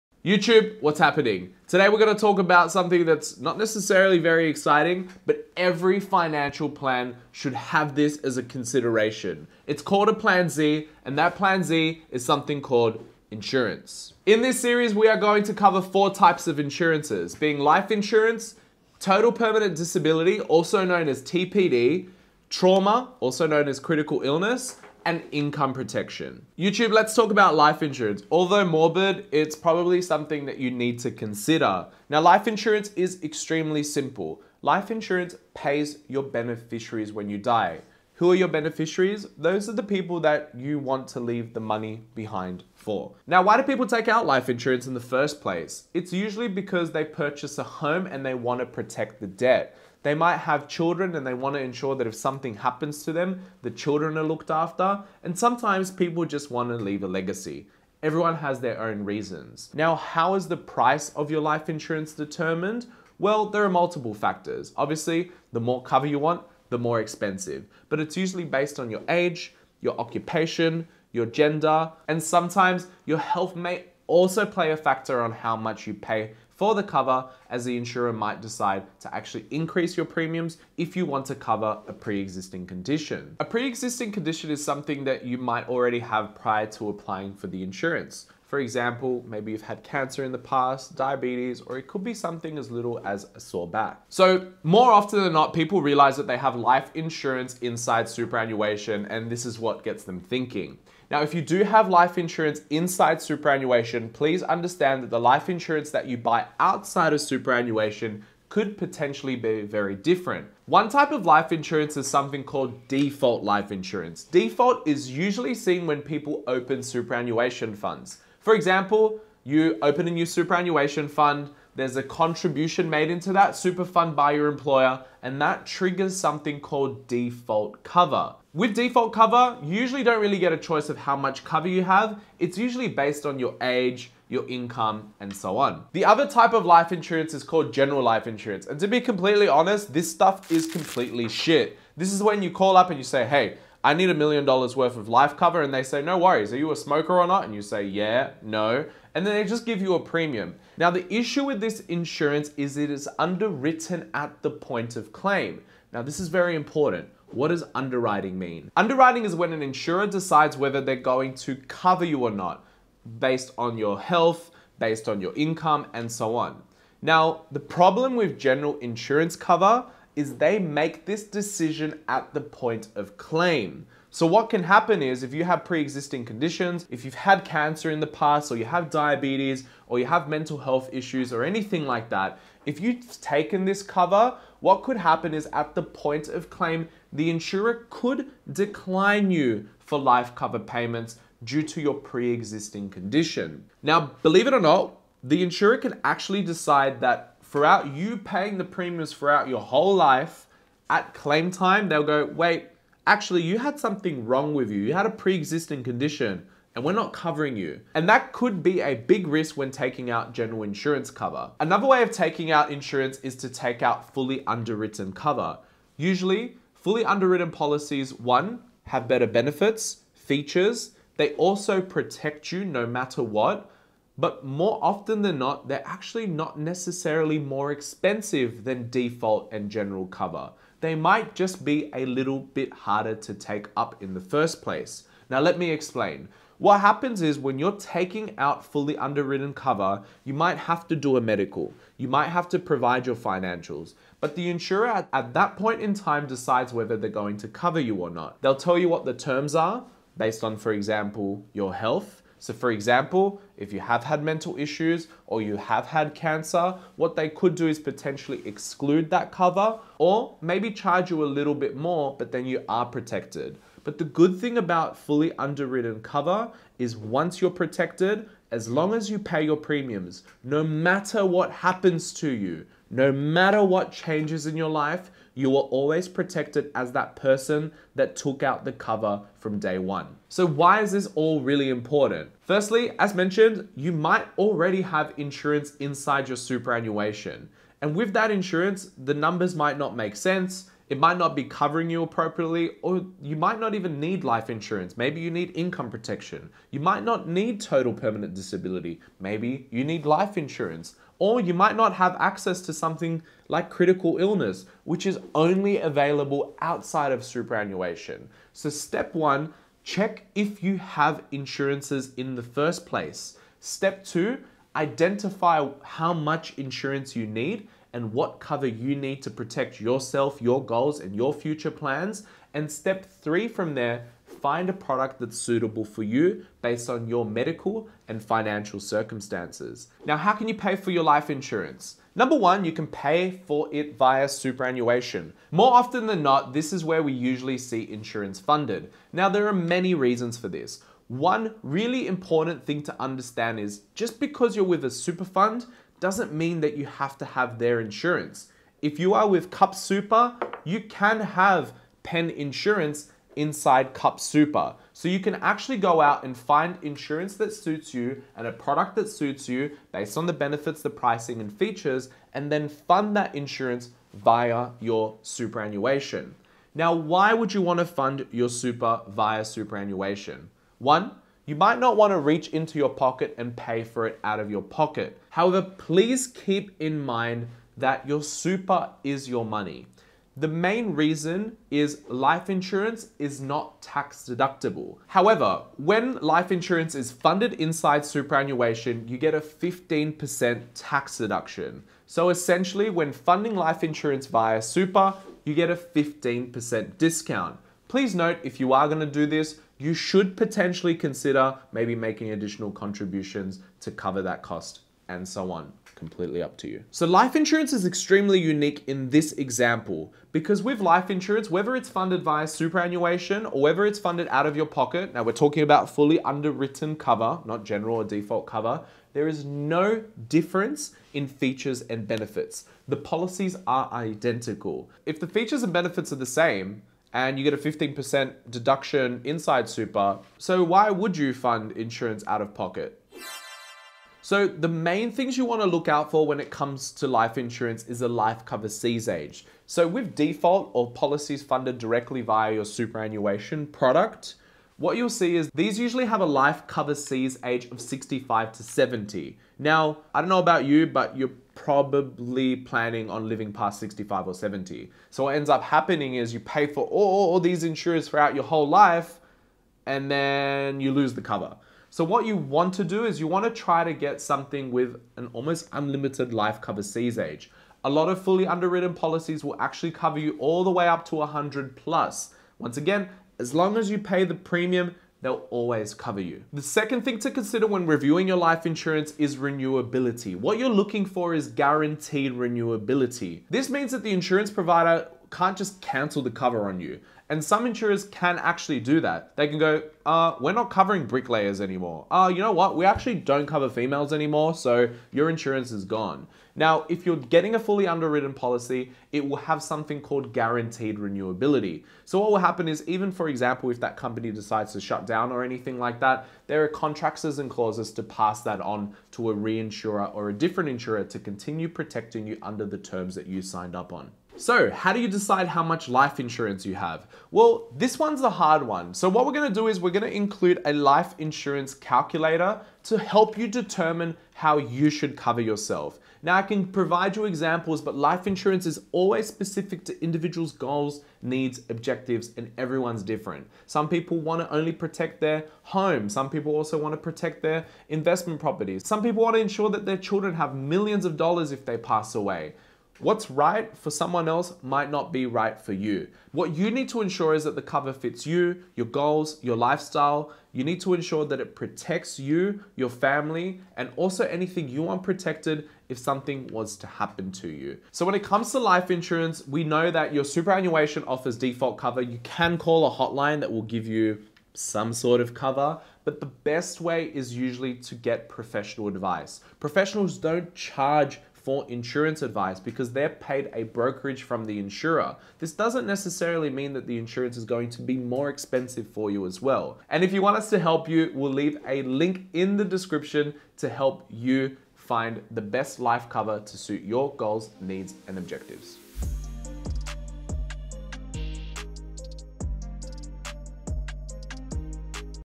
Life Insurance - Everything you need to know | Insurance Masterclass
How much cover do you need? Is it worth it? How much does it cost? Should you pay for it out of pocket or through super?

Everything you need to know is in this video and to dive deeper access the toolkit below.

🎥 VIDEO TEAM CREDIT

0:00 - Intro
0:27  - Life Insurance
1:00 - How much does it cost?
1:34 - Types of Life Insurance
5:08 - How to review your cover
5:30 - Funding / Payment Options
6:34 - Summary